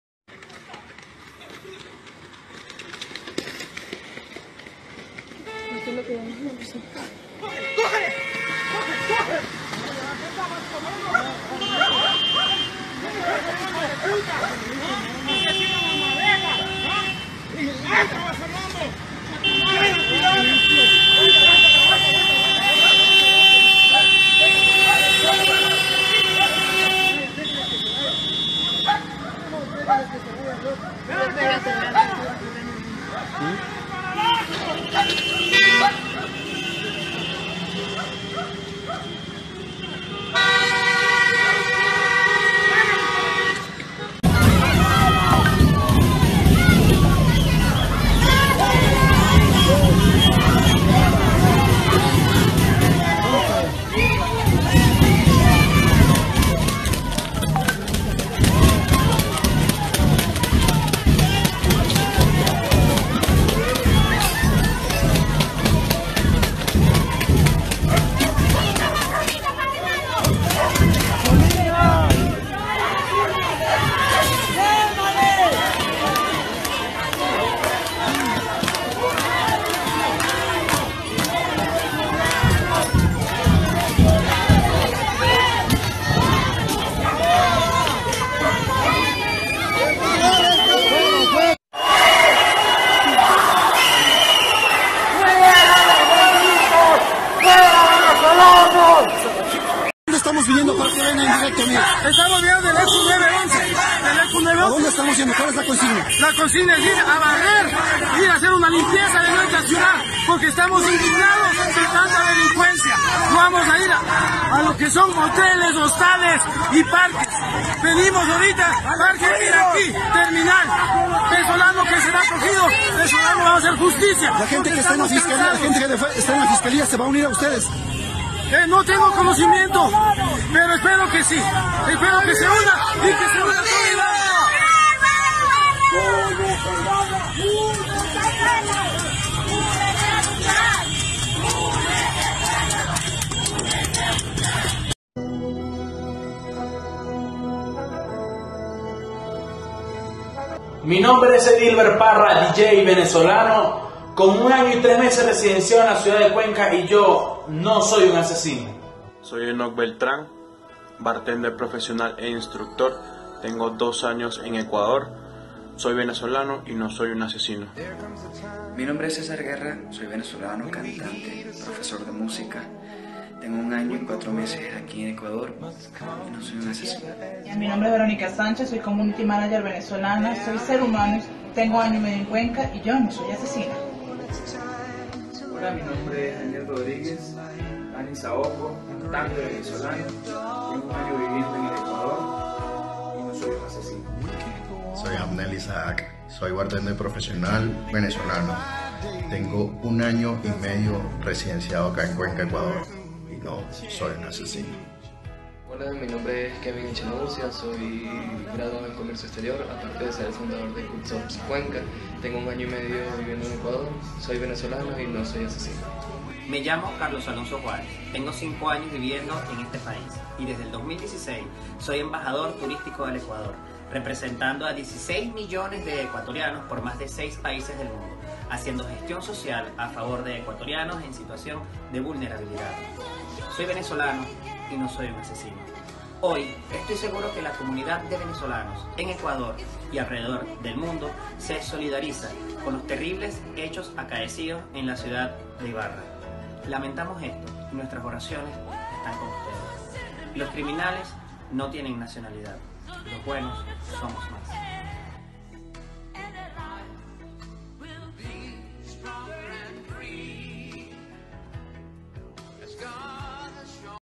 Ibarreños agreden refugiados venezolanos
Furibundo tumulto de ibarrenos, sale a las calles para expulsar de su ciudad a refugiados venezolanos, tras crimen atroz en el cual, un ciudadano de esa nacionalidad apuñaló a mujer embarazada.